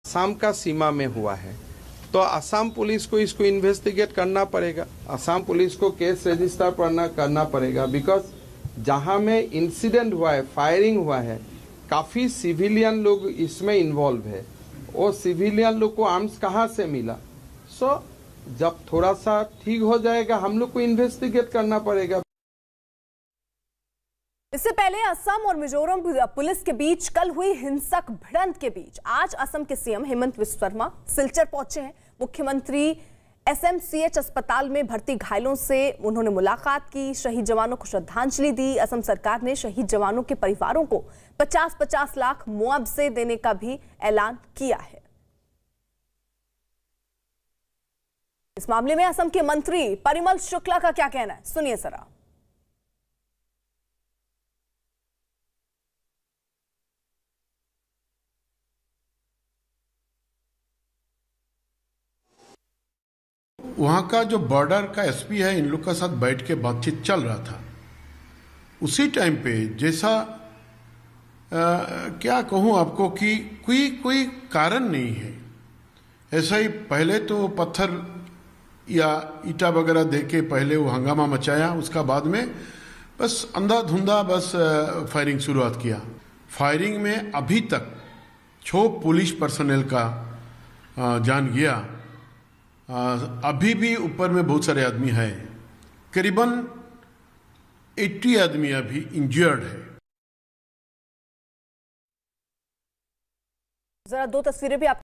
Assam-Mizoram Border पर हुई हिंसक झड़प पर क्या बोले Assam के CM Himanta Biswa Sarma ?
असम-मिजोरम सीमा पर हुई हिंसक झड़प में असम पुलिस के 5 जवानों की मौत हुई है, घटना के बाद असम और मिजोरम के बीच सीमा विवाद और गहरा गया है, गृह मंत्री अमित शाह ने मुद्दे को जल्द सुलझाने के निर्देश दिए हैं.इस हिंसा पर आज असम के CM क्या बोले? सुनिए

LIVE Hindi News | Kargil Vijay Diwas 2021| Maharashtra Floods | Breaking News | News18 India LIVE